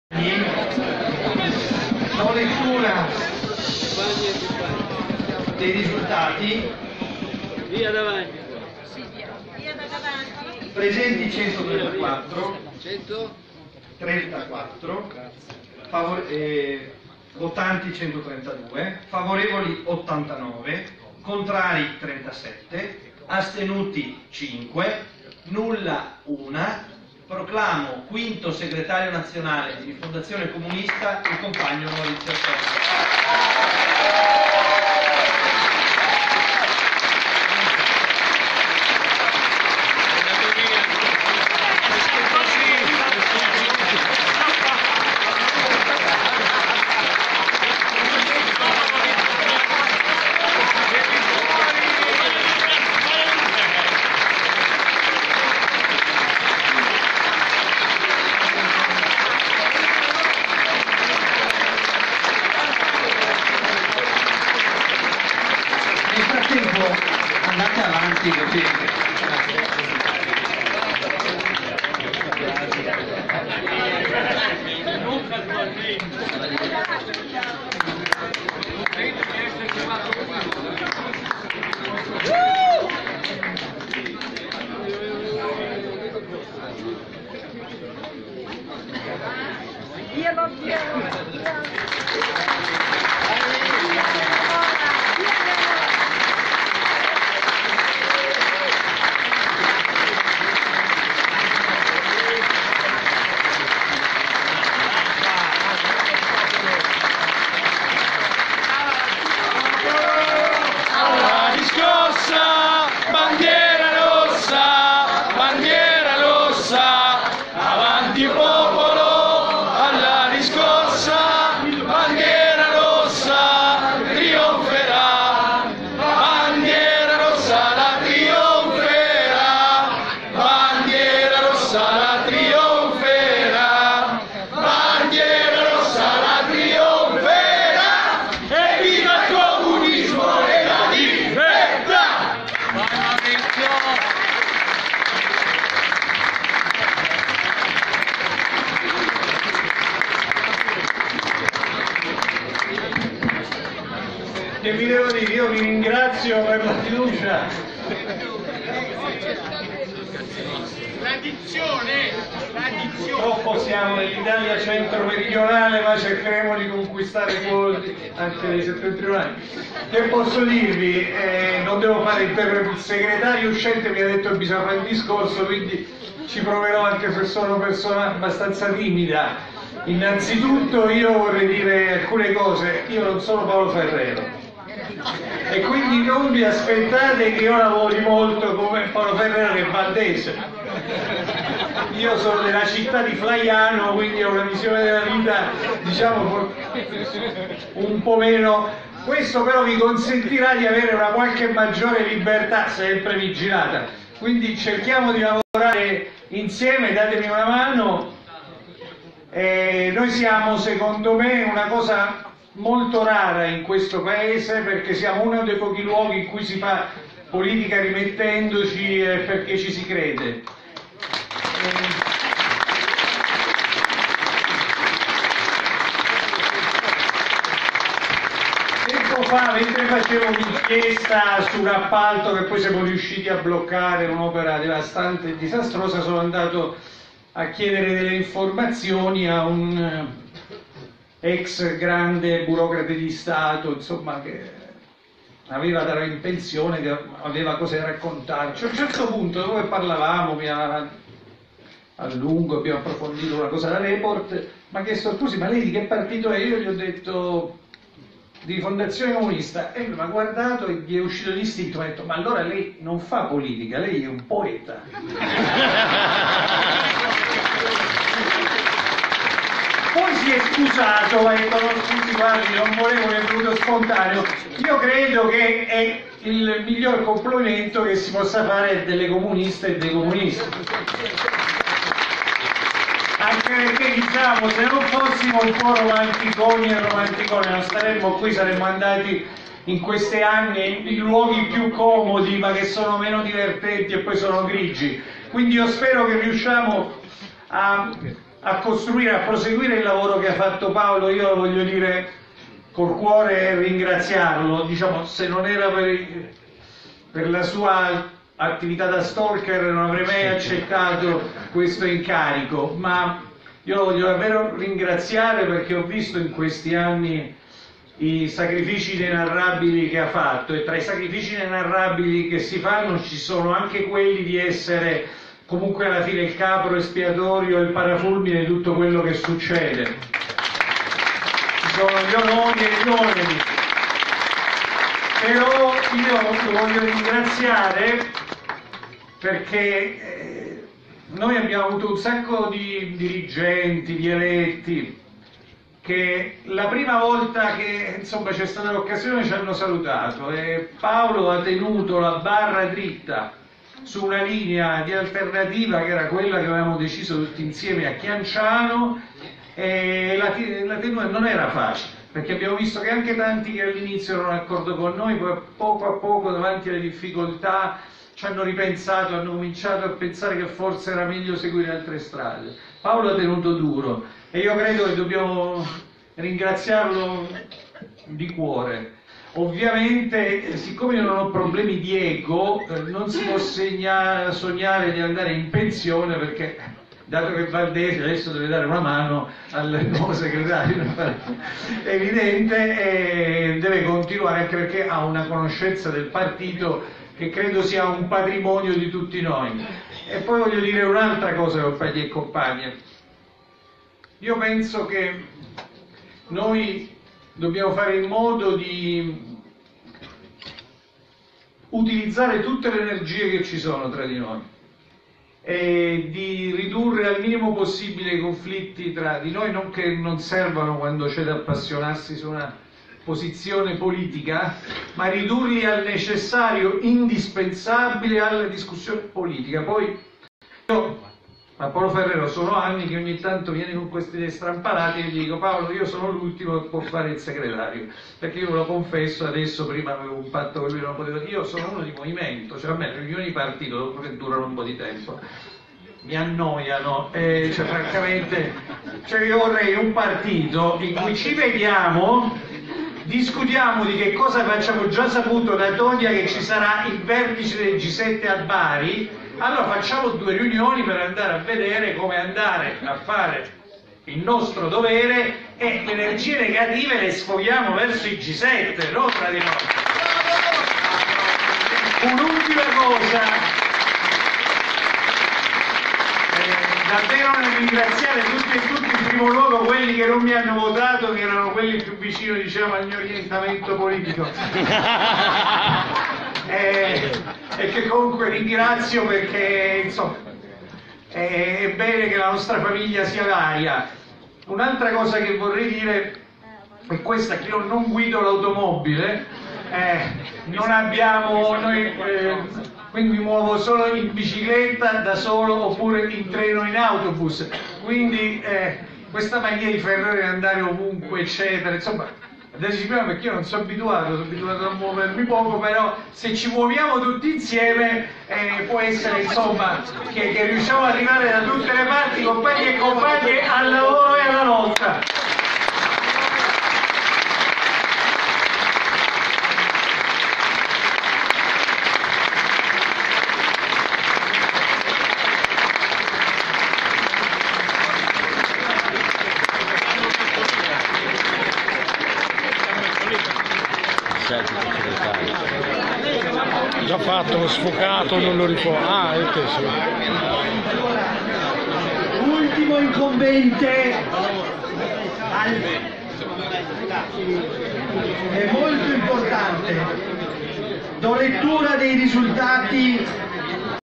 Maurizio Acerbo segretario del PRC - SE (FB video 02.04.2017)
X Congresso nazionale PRC - SE, Spoleto (PG), 2 aprile 2017. Maurizio Acerbo interviene dopo la sua elezione a segretario di Rifondazione Comunista (FB video)